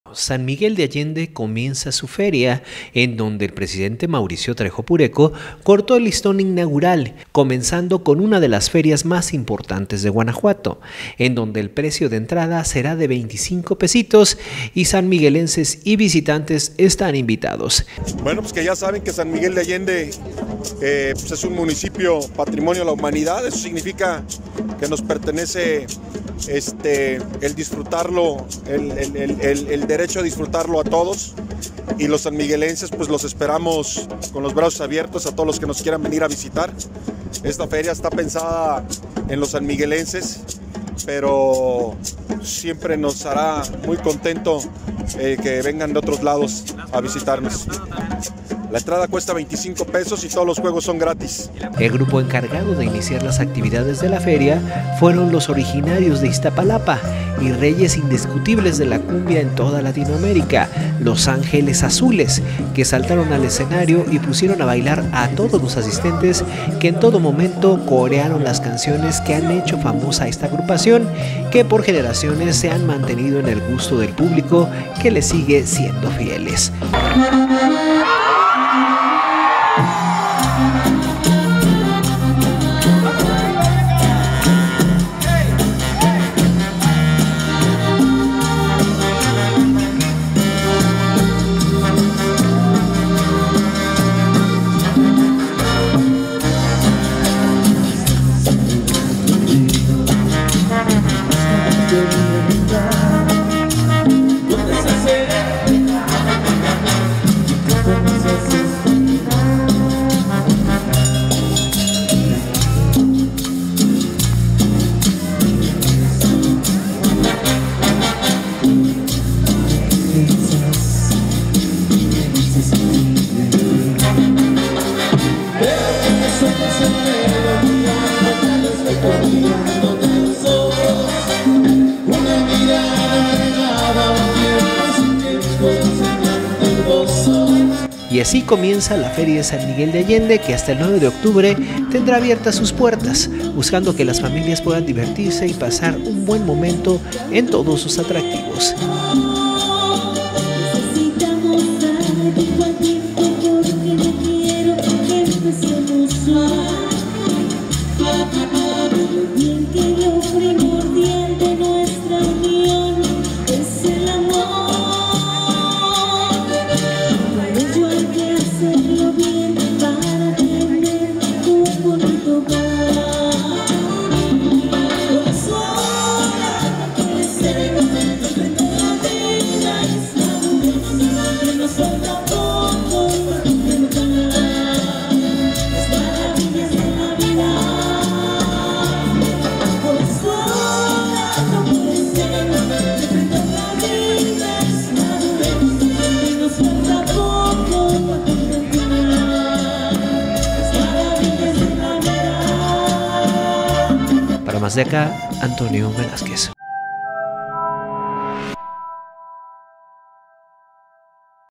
Inicia la feria de SanMiguelDeAllende bailando con los
FeriaSMA22 Inicia la feria de SanMiguelDeAllende bailando con los

La feria estará abierta de este 26 de septiembre y hasta el 9 de octubre, en donde se presentarán grandes artistas y se podrá disfrutar de los juegos, pabellones y demás atracciones que se han preparado para todo el que decida asistir